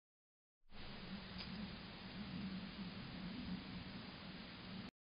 The gray fox family is still together
I assumed that Walker and Fluffy‘s baby had grown up and moved on to build his own family. I was wrong! They’re still together!